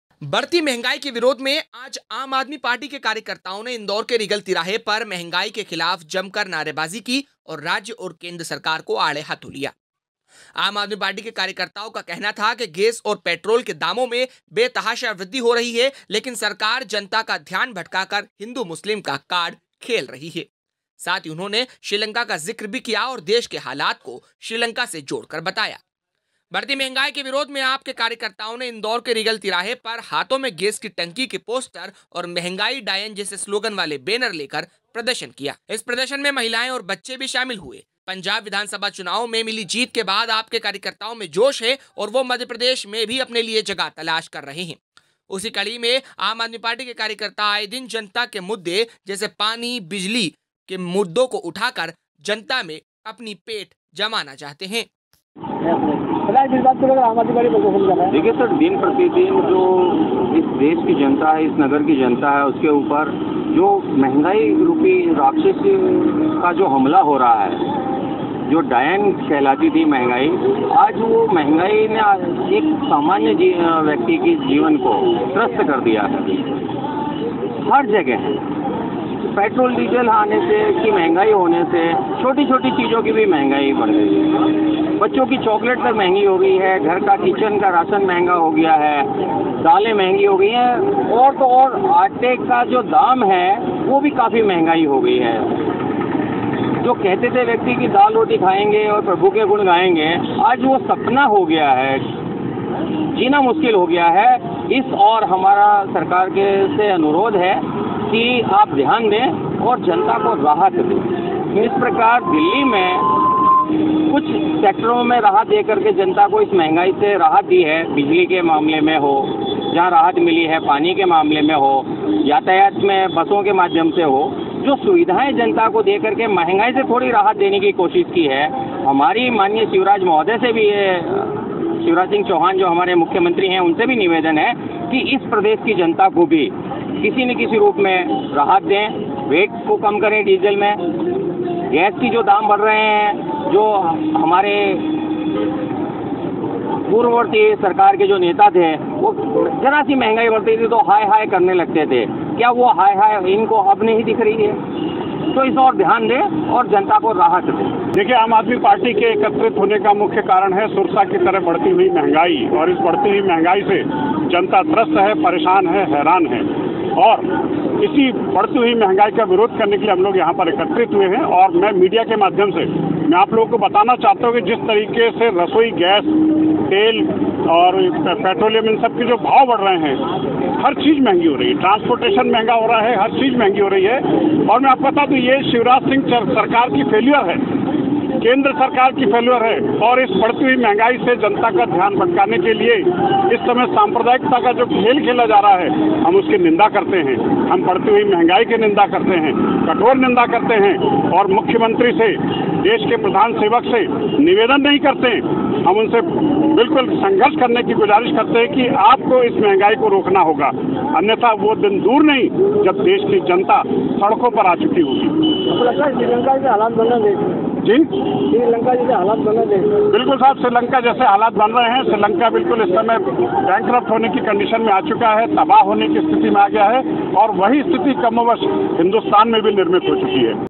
बढ़ती महंगाई के विरोध में AAP के कार्यकर्ताओ ने इंदौर के रीगल तिराहे पर प्रदर्शन किया
इंदौर बढ़ती महंगाई के विरोध में आज आम आदमी पार्टी के कार्यकर्ताओ ने इंदौर के रीगल तिराहे पर मंहगाई के खिलाफ जमकर नारे बाजी की ओर राज्य और केंद्र सरकार को आड़े हाथों लिया देश मे आये दिन गेस ओर पेट्रोल के दामो में बेतहाशा मूल्य वृद्धि हो रही है और सरकार जनता का ध्यान भटकाकर हिन्दू मुस्लिम का कार्ड खेल रही है साथ ही उन्होंने श्रीलंका का जिक्र भी किया और देश के हालत को श्रीलंका से जोड़कर बताया